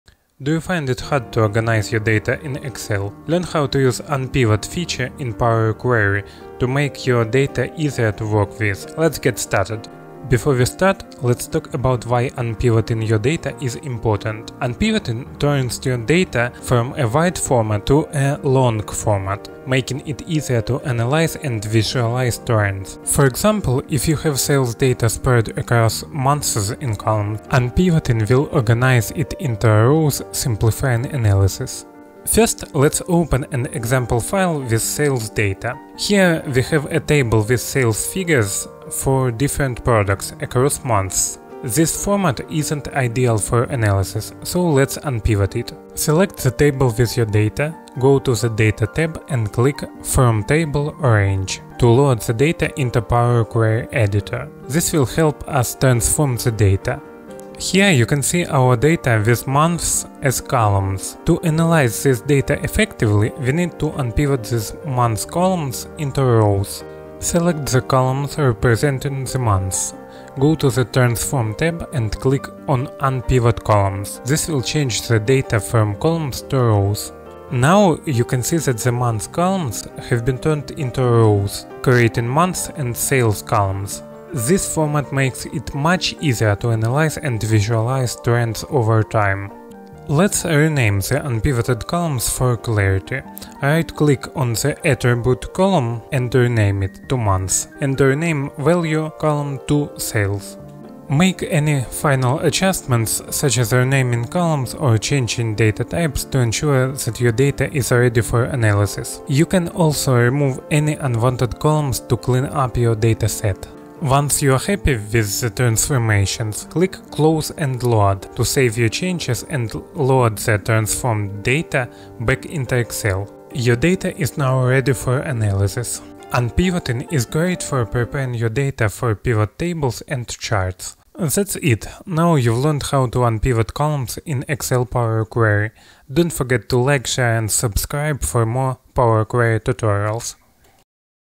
Unpivot Data in Excel - Power Query Basics Tutorial
Are you struggling to organize your data in Excel? In this tutorial, we will show you how to use the unpivot feature in Excel Power Query to transform your data from a wide format to a long format, making it much easier to analyze and visualize trends.

In this video, you will learn:
    The importance of unpivoting data and why the long format is better for analysis
    How to load your data into Power Query
    Step-by-step instructions on unpivoting columns
    The different unpivot options available in Power Query
    How to rename columns and make final adjustments for clarity
    Tips for saving and using your transformed data in Excel

Timestamps:
00:00 - Intro
00:11 - Why is Unpivoting Important?
00:32 - Open and Load the Data to Power Query
01:01 - Unpivoting Columns
01:33 - Rename Columns
01:54 - Close and Load